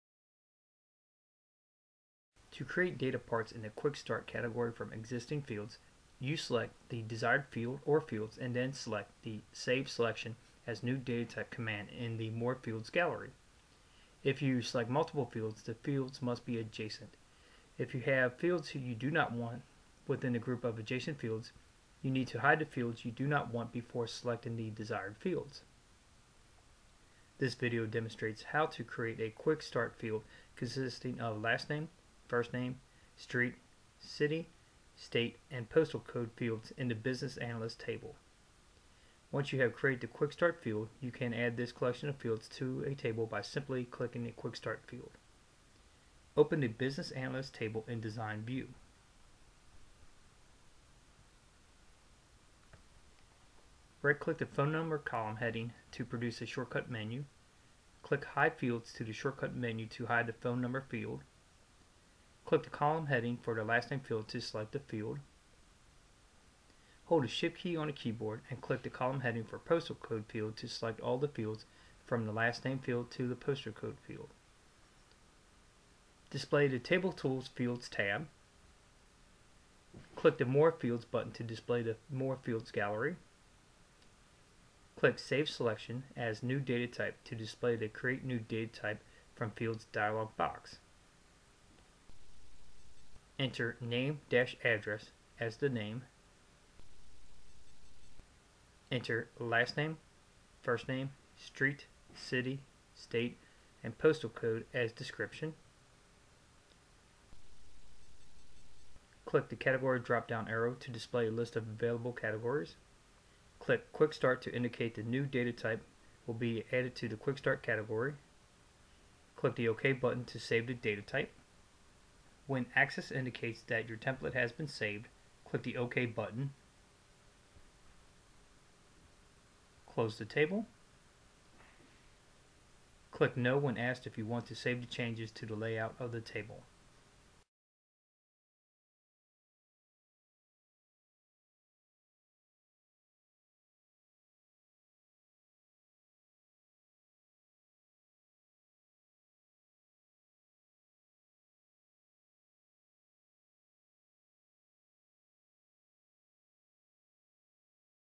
Microsoft Access: Create Custom Data Parts
This brief video walks you through creating custom data parts.